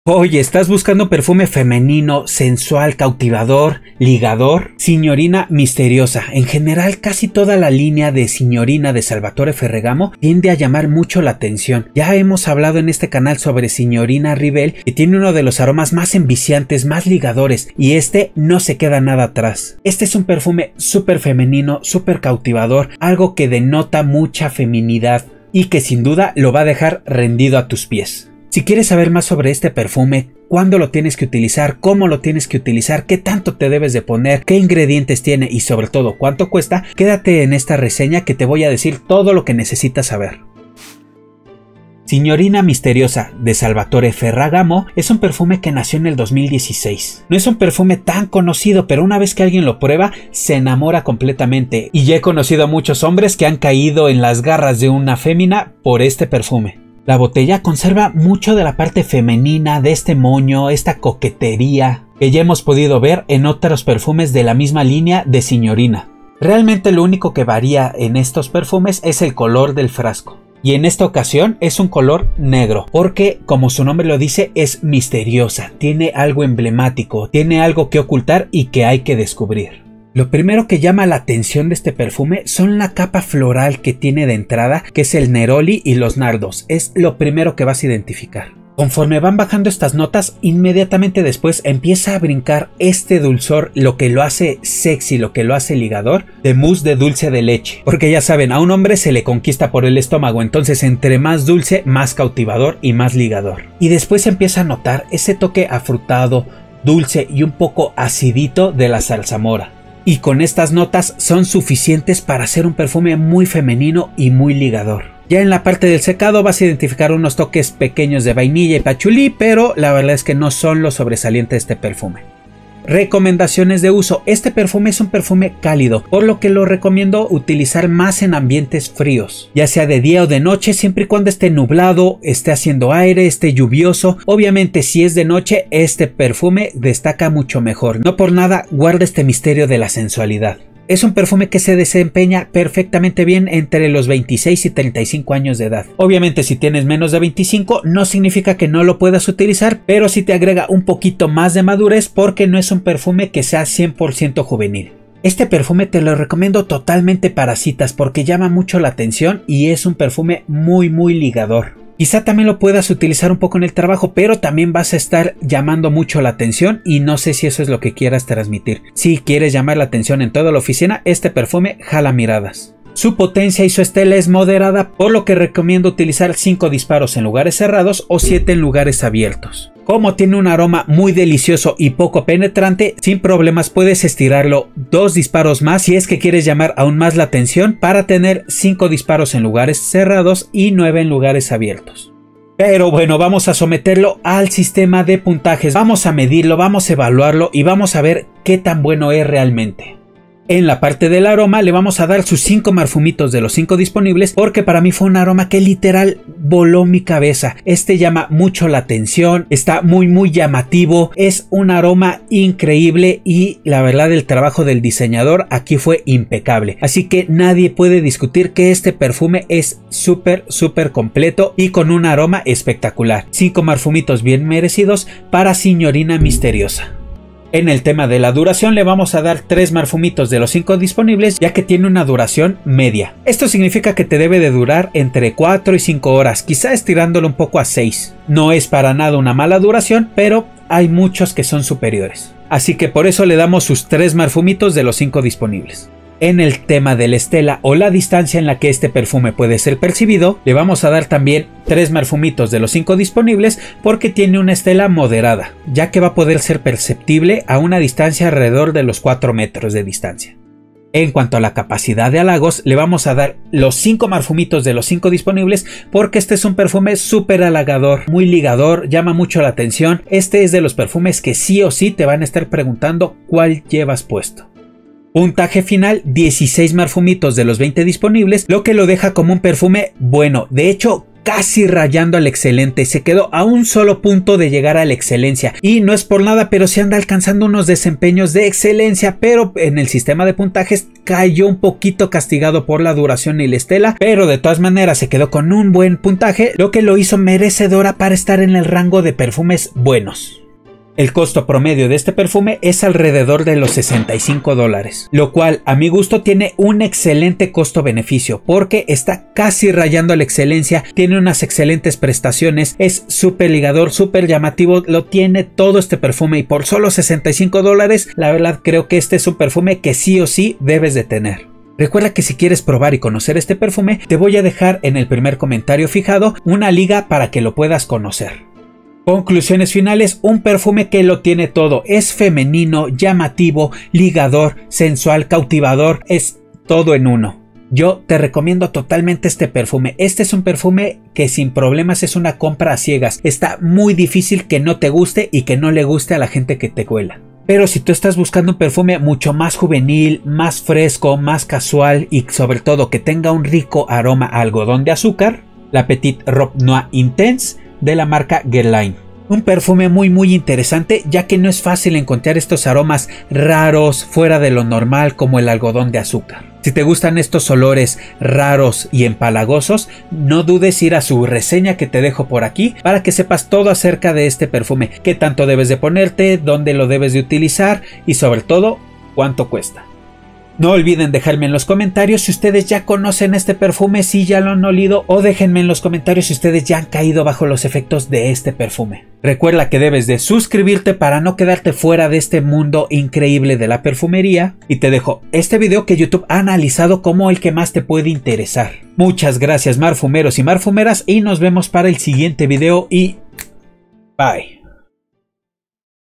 🤩DESLUMBRA a los HOMBRES 👨‍💼 con este LIGADOR Aroma 💕| Salvatore Ferragamo Signorina MISTERIOSA ❗️
En General TODA la Linea Singnorina del Diseñador Salvatore Ferragamo son perfumes FEMENINOS que recomiendo MUCHÍSIMO Tener, y Singorina Misteriosa es una compra OBLIGADA 🥰.

Un SUPER ligador Aroma que vuelve locos a los HOMBRES, y que JAMAS te Fallará

Si quieres saber más de este perfume, quédate en esta reseña, donde te contaré TODO

00:01 Introducción signorina misteriosa salvatore ferragamo reseña
00:42 ¿Cuándo salió signorina misteriosa salvatore ferragamo?
01:20 Aroma. ¿Cómo huele el perfume signorina Misteriosa?
02:03 Recomendaciones de Uso. Salvatore Ferragamo Signorina MISTERIOSA
03:12 Puntuación  ¿Qué tal huele el signorina Misteriosa?
05:00 Signorina misteriosa salvatore ferragamo Precio
05:29 Conclusión: signorina misteriosa salvatore ferragamo review